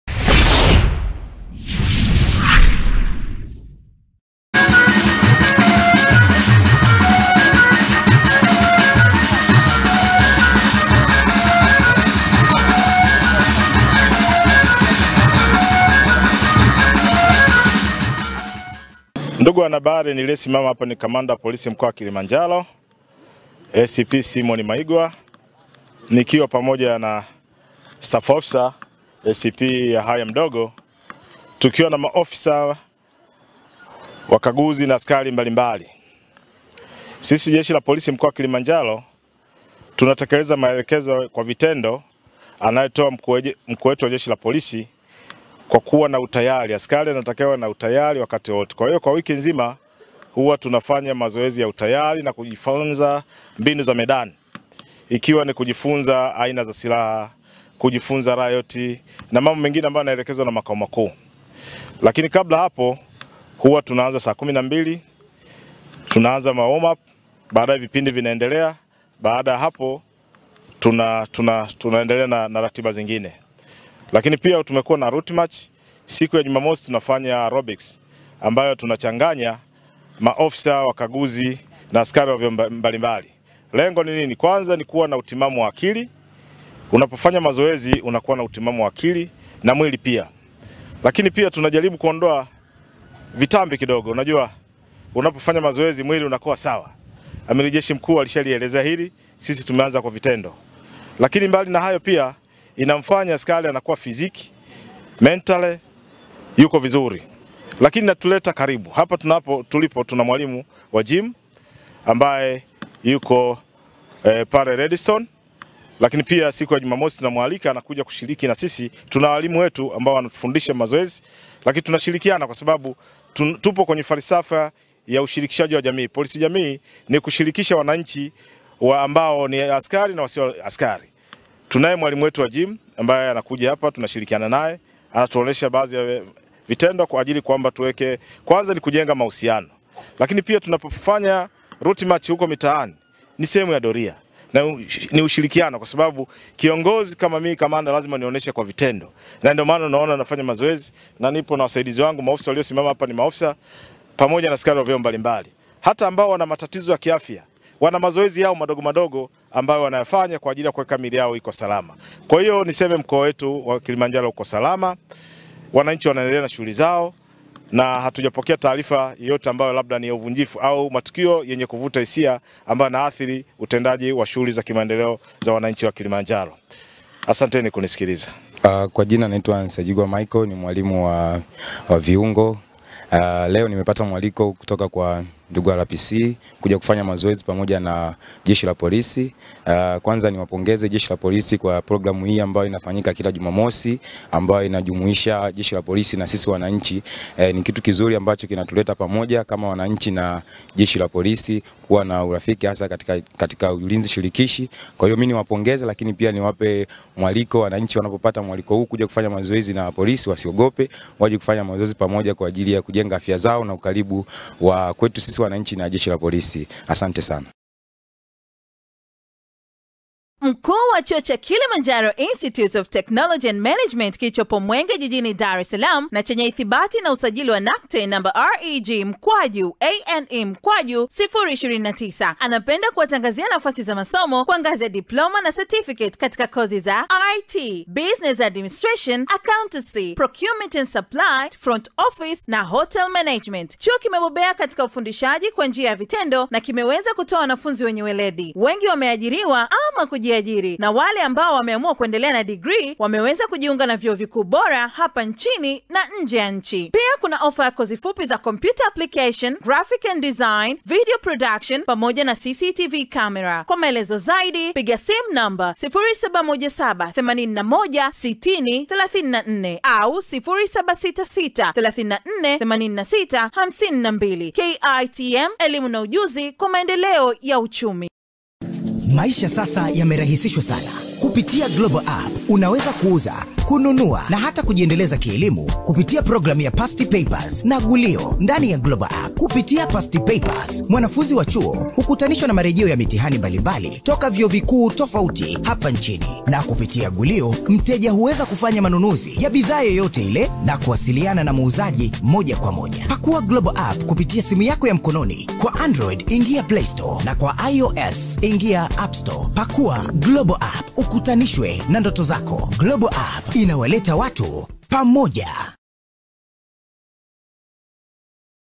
AGIZO LA IGP WAMBURA LATEKELEZWA KILIMANJARO, POLISI WAANZA KUFANYA MAZOEZI YA UKAKAMAVU..
JESHI la Polisi Mkoani Kilimanjaro limetii agizo la Rais Samia kwa kufanya mazoezi ya kutoa vitambi na kujenga afya.

Kamanda wa Jeshi la Polisi Mkoani humo amezungumza kwa undani..